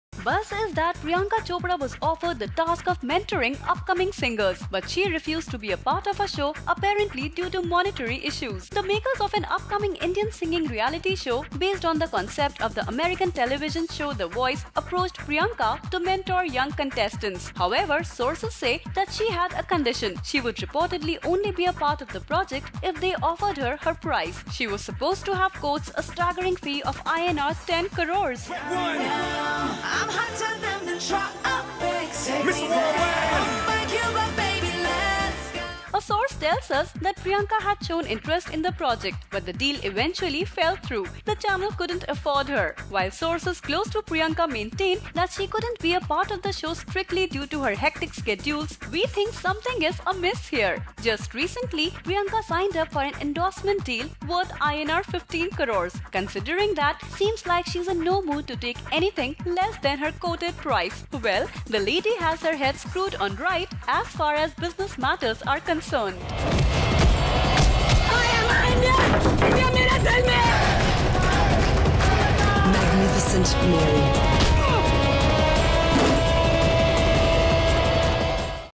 Money matters to Priyanka
Priyanka Chopra is all set to fly off to Los Angeles for a duration of three months to begin shooting for her deal with Modern Family fame ABC television studio! The Bajirao Mastani actress will be staying in LA for three months and the ABC show she’ll feature in will revolve around PC, she being the central character. While my interest is piqued to find out more details about Priyanka’s international show, what’s left my mind boggled is the amount of money Priyanka is getting to do the ABC show! Rs 25 crore! That’s right!